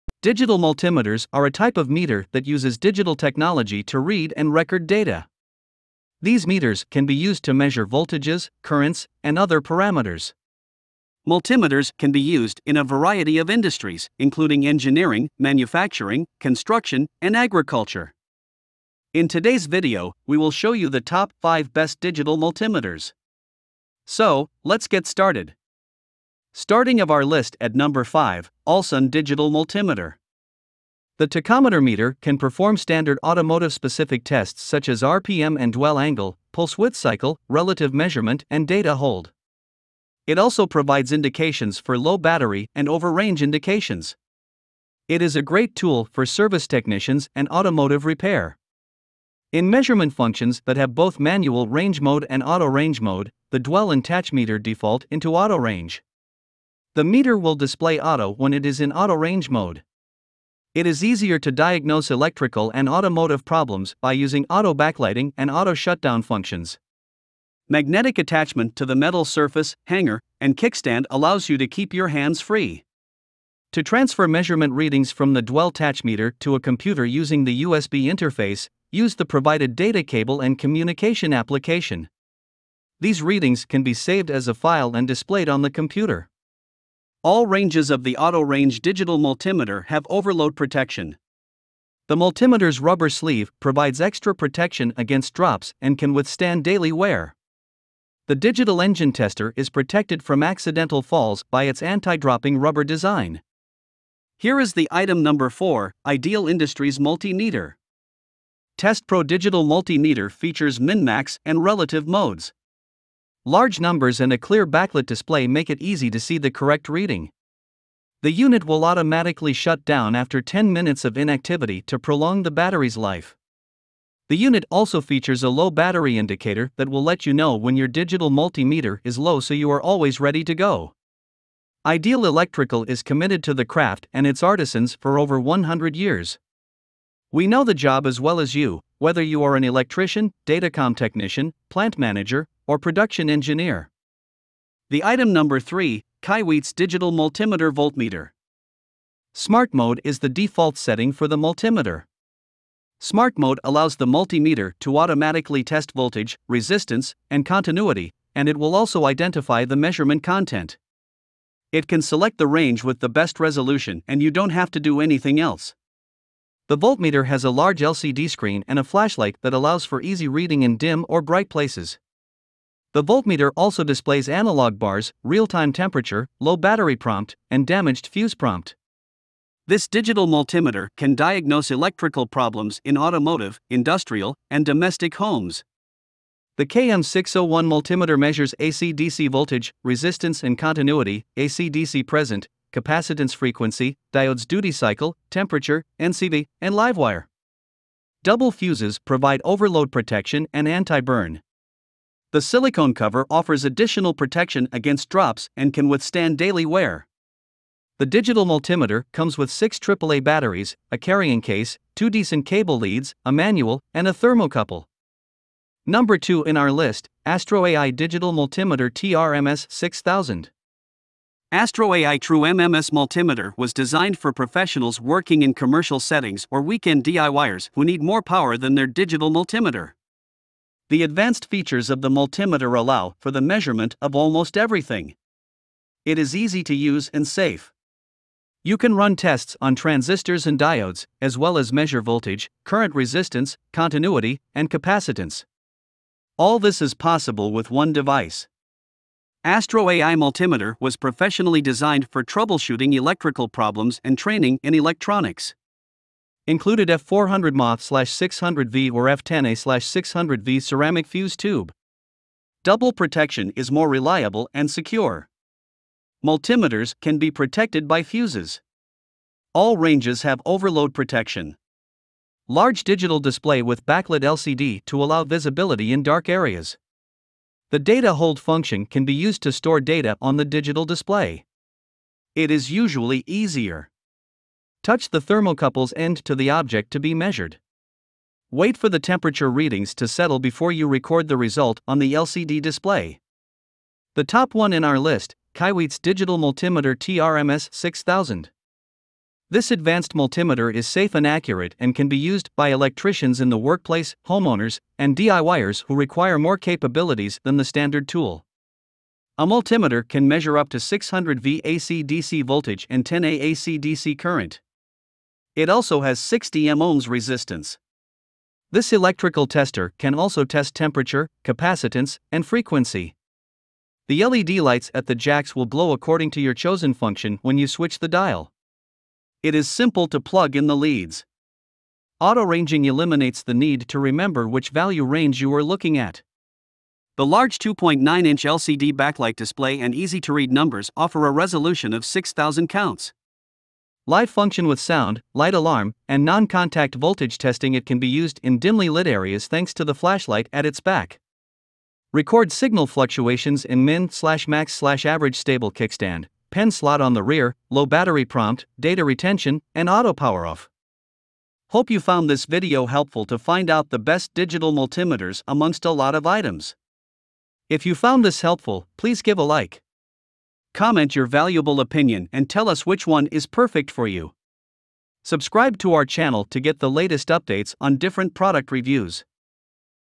Top 5 Best Digital Multimeter 2022
5. Allsun Digital Multimeter

4. Ideal Industries Multi-Meter

3. KAIWEETS Digital Multimeter Voltmeter

2. AstroAI Digital Multimeter TRMS 6000

1. KAIWEETS Digital Multimeter TRMS 6000